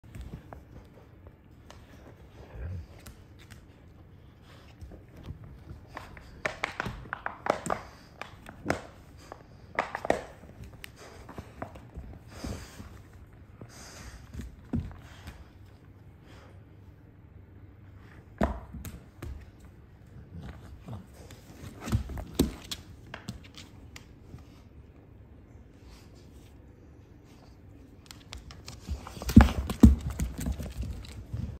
"I am just going to lie on the couch while you try to get the toy", the ultimate couch showdown 😊😊😊
Big Papa Hermes shows XL American Bully puppy Princess Kleo how tag-of-war is done the lazy way, one sneaky move and he runs off with the toy 😊😊😊